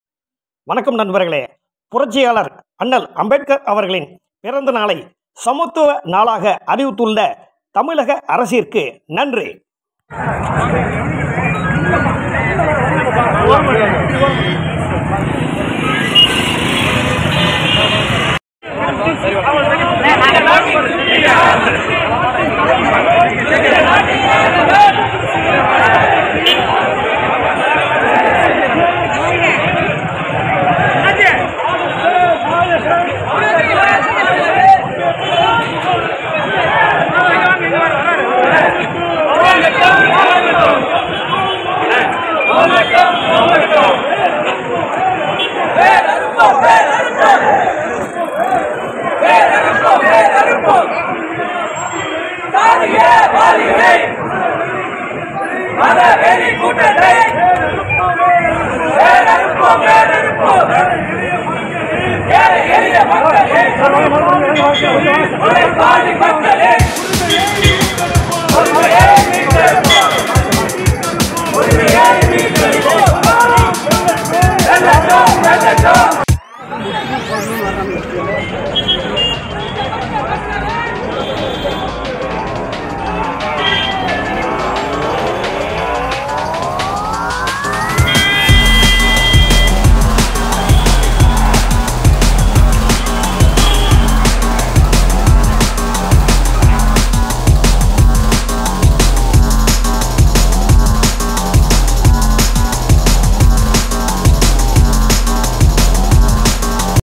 equality_day | திருச்சி மத்திய பேருந்து நிலையத்தில்..|| G Pay 9443645065 ~For your Contribution...
வணக்கம்
திருச்சி மத்திய பேருந்து நிலையம் அருகே உள்ள அரிஸ்டோ ரவுண்டானாவில் அமைந்துள்ள..சட்டமேதை .அண்ணல் அம்பேத்கர் பிறந்த நாள் விழா ~ காணோலி..பதிவு..சமத்துவநாள் ..மகிழ்வில்..தினம் விஷன்..